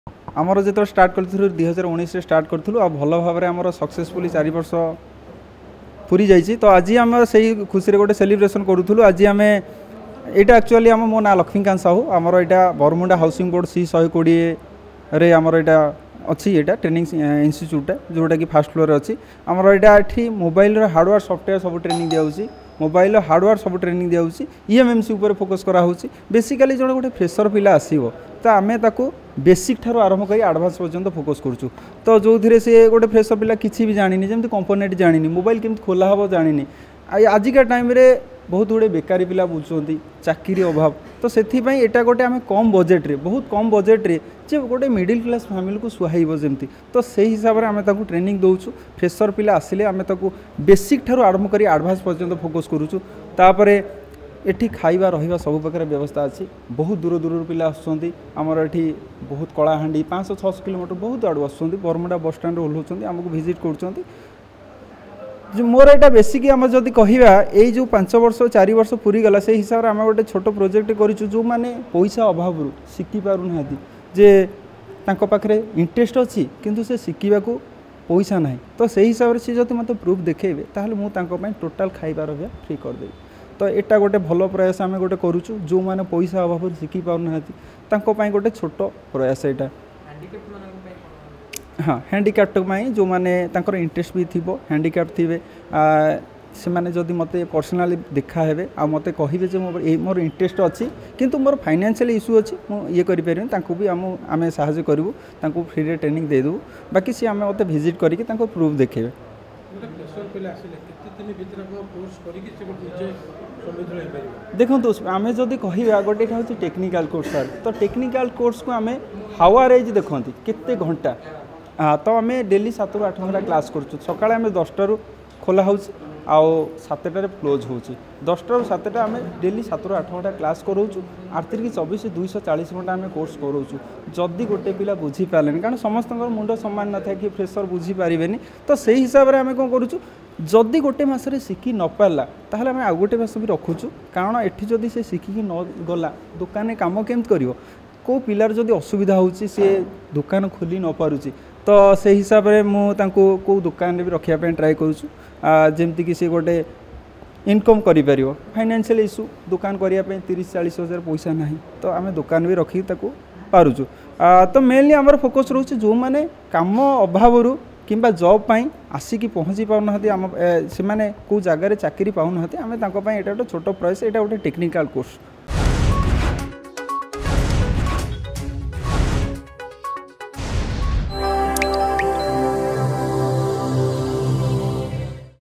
Free Mobile Repairing Training For Needy Candidates In Bhubaneswar | Argus News
Free Mobile Repairing Training For Needy Candidates In Bhubaneswar